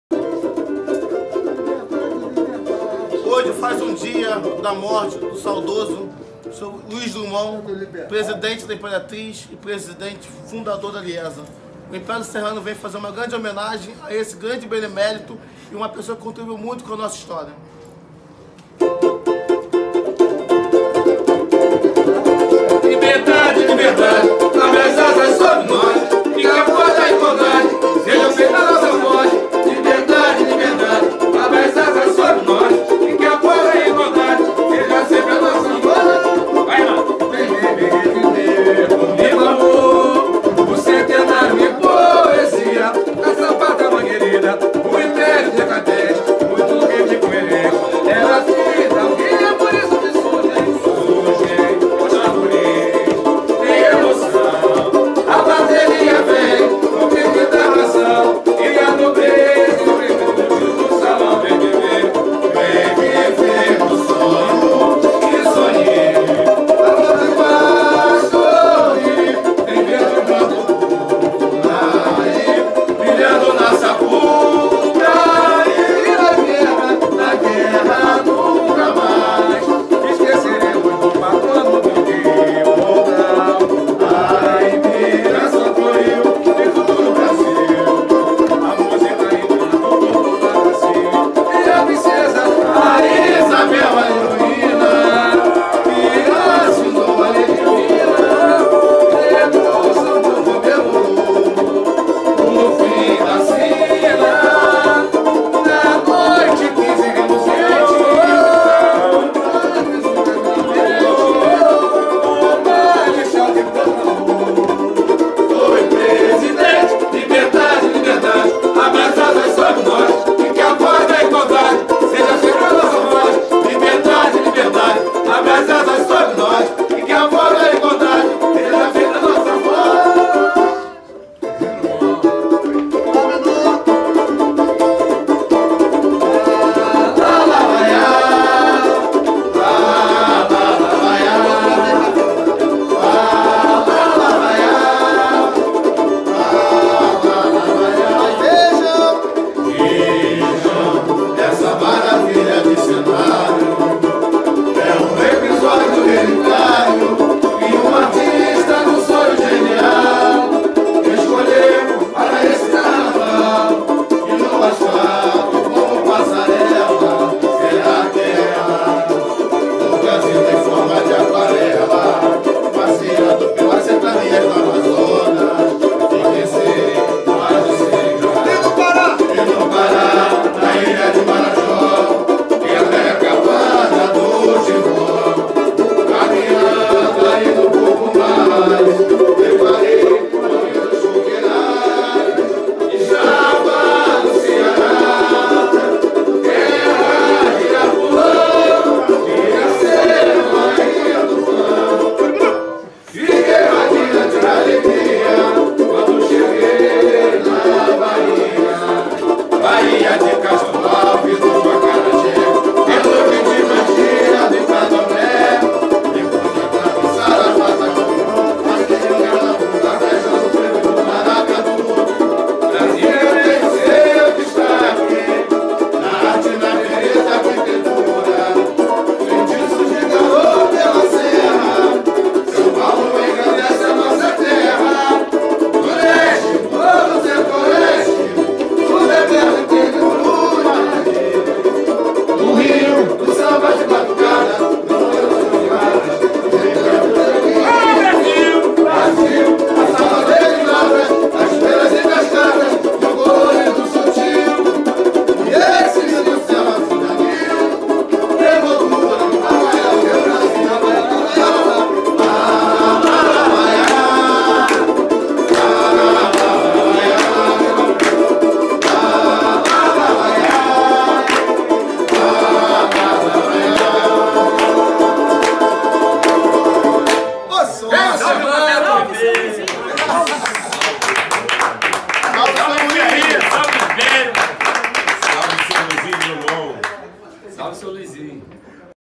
Império Serrano faz homenagem ao Sr. Luizinho Drumond !
Créditos : TV IMPÉRIO SERRANO

Presidente Sandro Avelar com  Alex Ribeiro, Xande de Pilares e Renato da Rocinha prestam uma singela homenagem ao saudoso benemérito do Império, o Sr Luiz Pacheco Drumond.

》👍🏻 Curtem e Compartilhem  ▶ Aqui o Carnaval é o Ano Inteiro 🎉🎊🥇